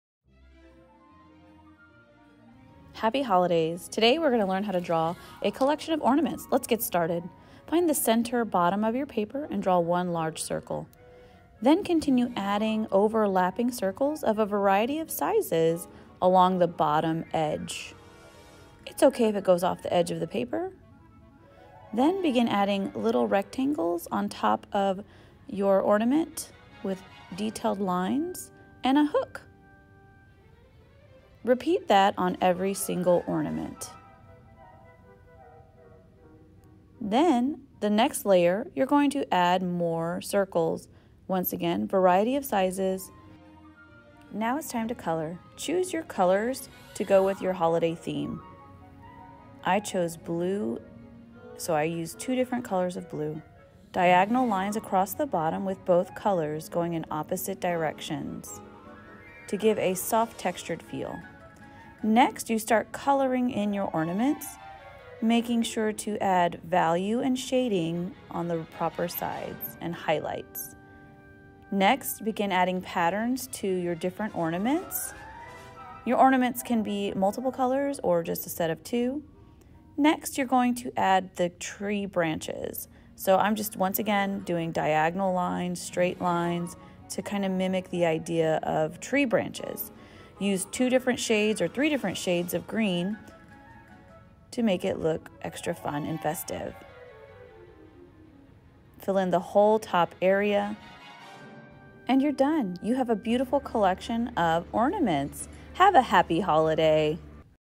Easy & Fun Ornament Drawing Tutorial
Draw or paint a collection of Ornaments just in time for the Holiday!